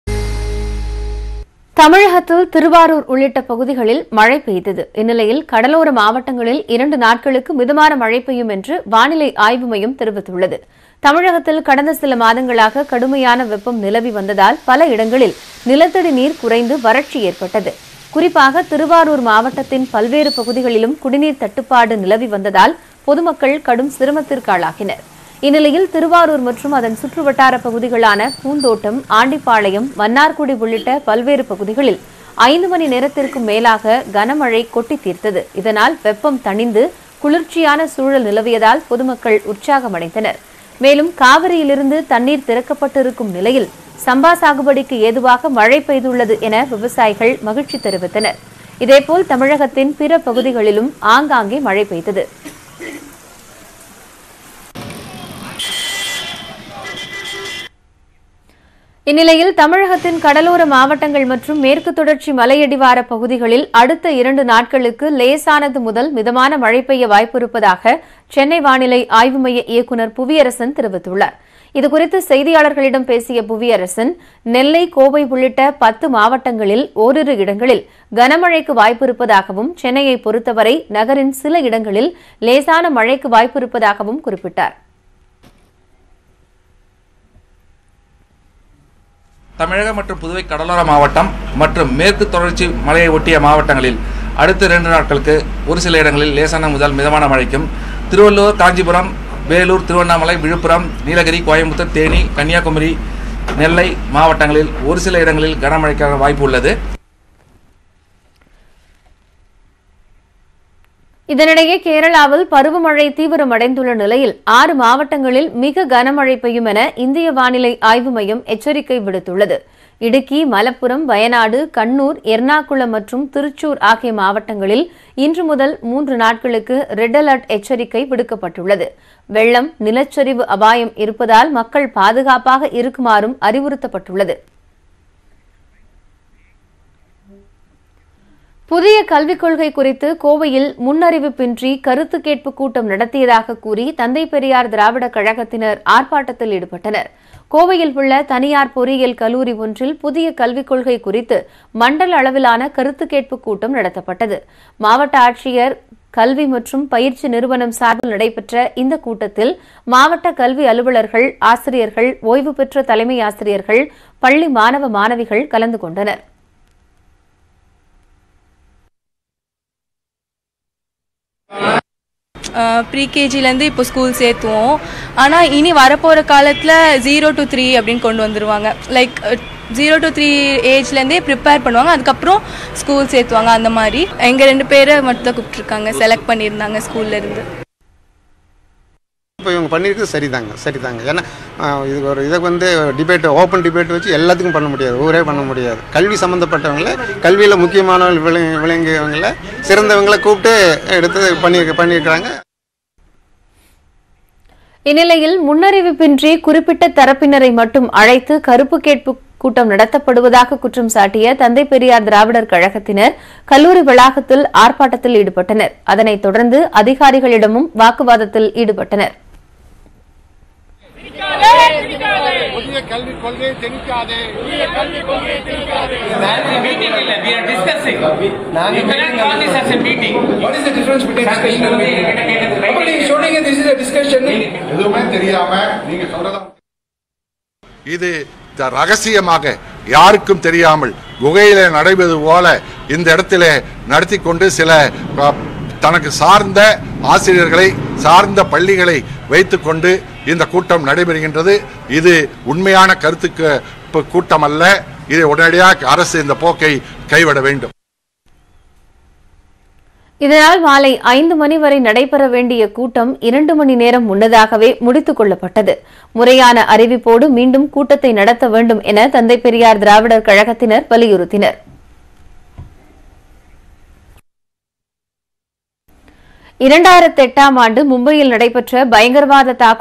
கடலோர மாவட்டங்களில் இரண்டு நாட்களுக்கு மிதமான மழை பெய்யும் - வானிலை ஆய்வு மையம்
தமிழகத்தில் திருவாரூர் உள்ளிட்ட பகுதிகளில் மழை பெய்தது, இந்நிலையில் கடலோர மாவட்டங்களில் இரண்டு நாட்களுக்கு மிதமான மழை பெய்யும் என்று வானிலை ஆய்வு மையம் தெரிவித்துள்ளது